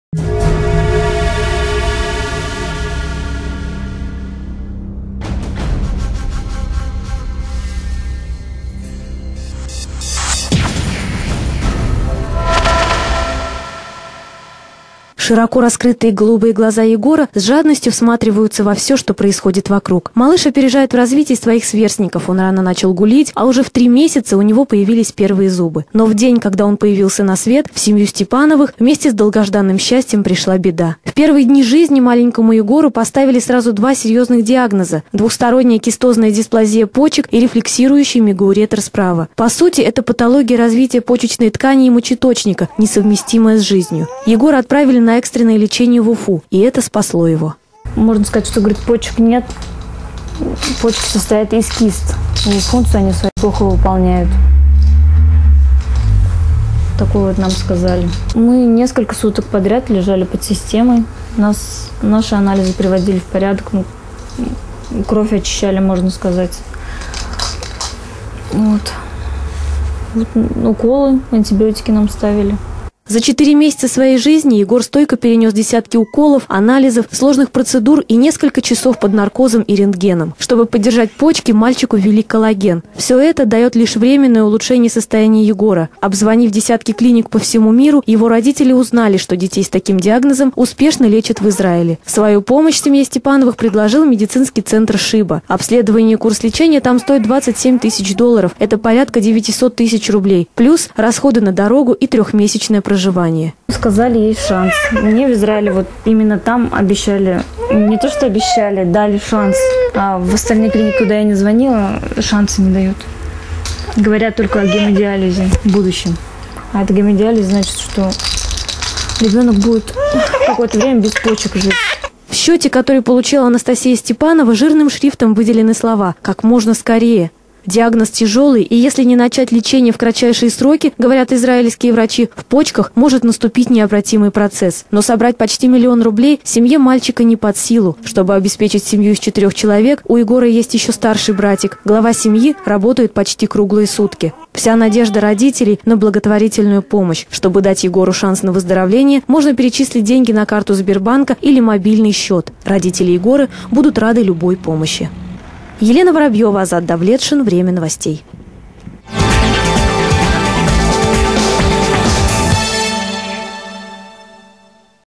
Поможем Егору!
В последнее время в нашу редакцию все чаще обращаются родители, чьим детям требуется дорогостоящее лечение. Наш сюжет о четырехмесячном Егоре, который с первого дня своей жизни борется с  серьезным заболеванием. Чтобы помочь мальчику, нужно ни много ни мало 1 миллион рублей.